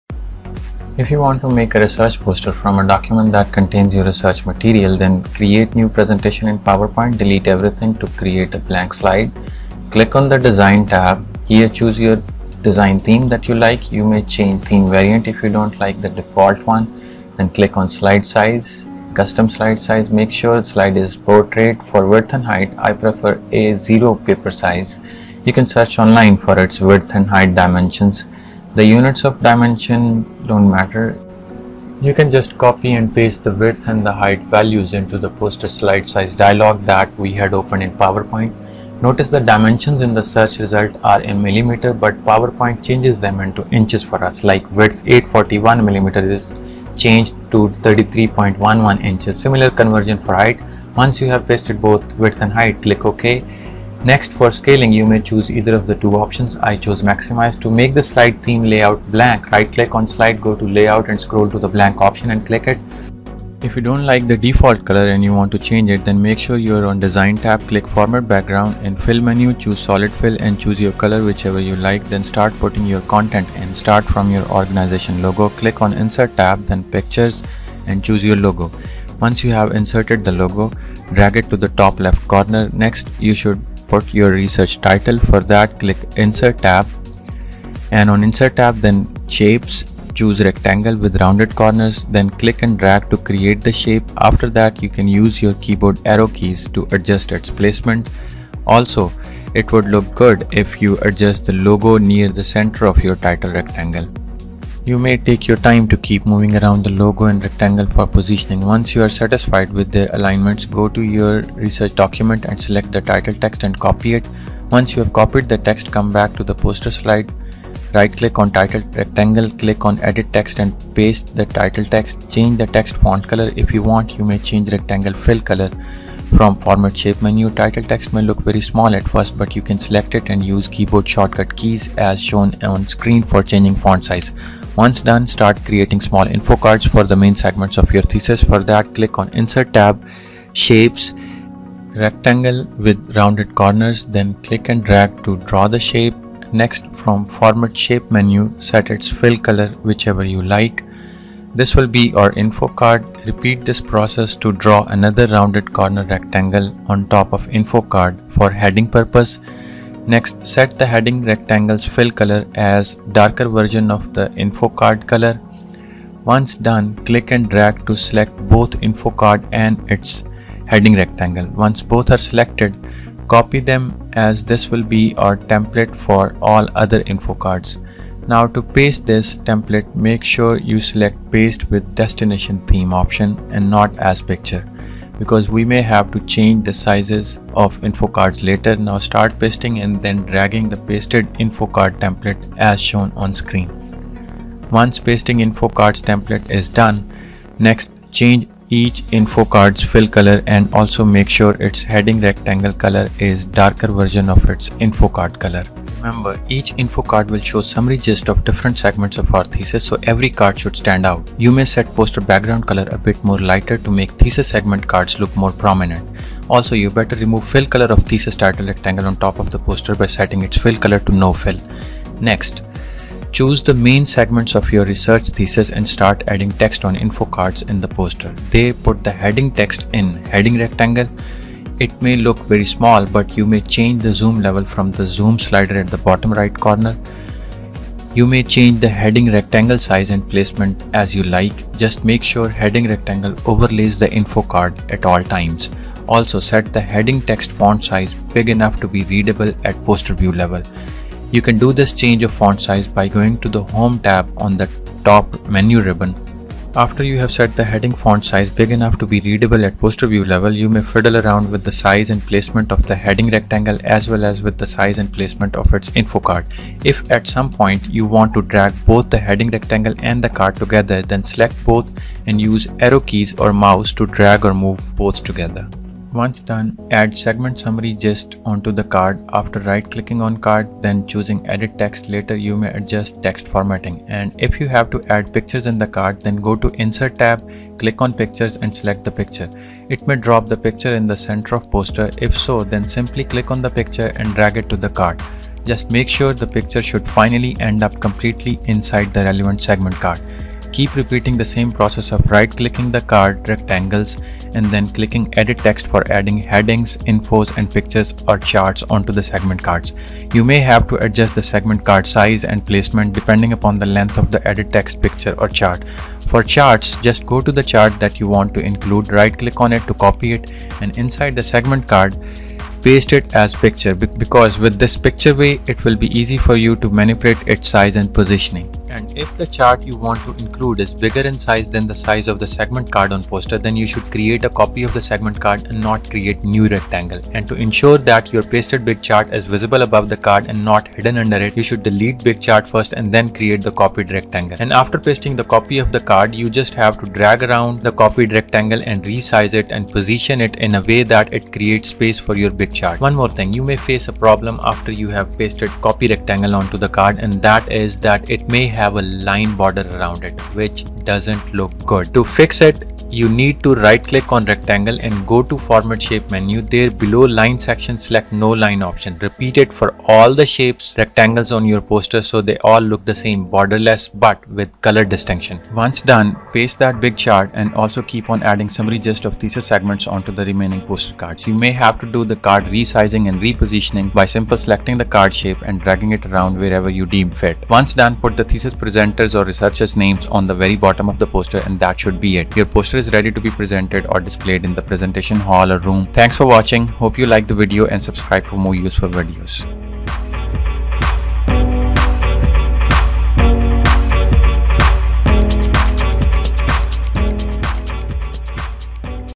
How To Create Academic Poster in PowerPoint || Research Poster in PowerPoint || Tutorial
Fonts used:
For the Title of Research on the very top:
Font Name=Calibri(Body)
Font Size=80
Bold
For thesis segment card headings text:
Font Name=Times New Roman
Font Size=44
Bold
For thesis segment card detail text:
Font Size=32
Normal/Regular (Not Bold)
For Research Author names at the very bottom:
Font Size=44
Bold